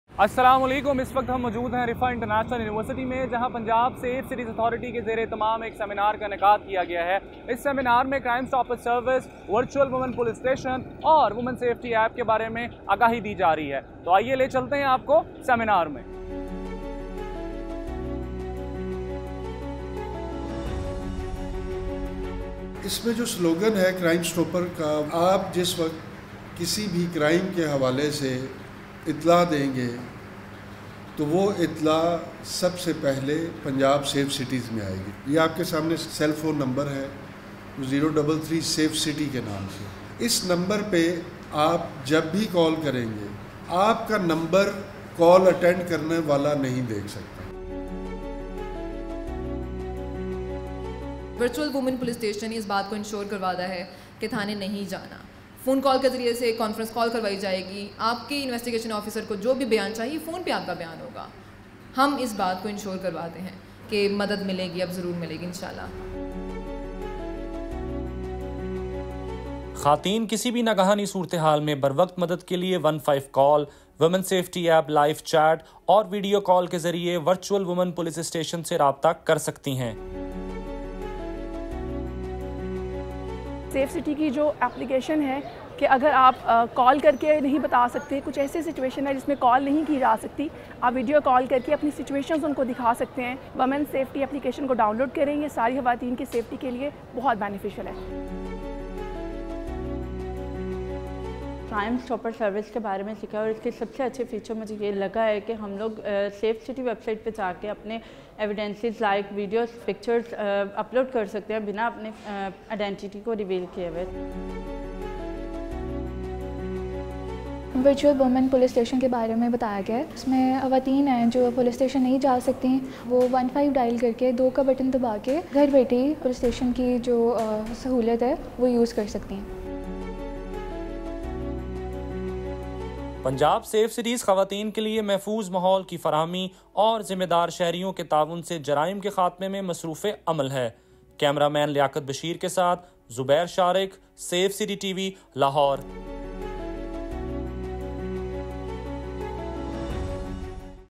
Insights into Pakistan's First Virtual Police Station & Crime Stopper Service | Seminar Recap
An awareness seminar on Pakistan’s first ever "Virtual Women Police Station", "Women Safety App" and "Crime Stopper Service" was organized at Rifa International University under the aegis of Safe Cities Authority. The participants were briefed about the working of Virtual Women Police Station, Women Safety App and Crime Stopper Service. Details in this special report